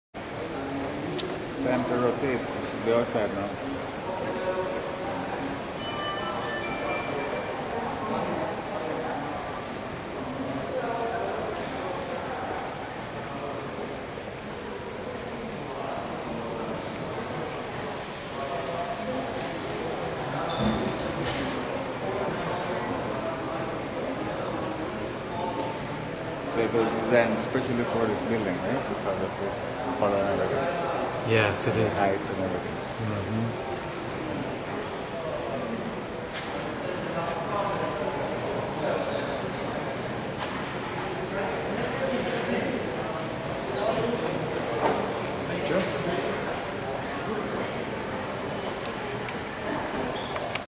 Long Island Jewish Medical Center Clock, by Timeshapes
Long Island Jewish Medical Center Clock (zooming in across the lobby)

Timeshapes.com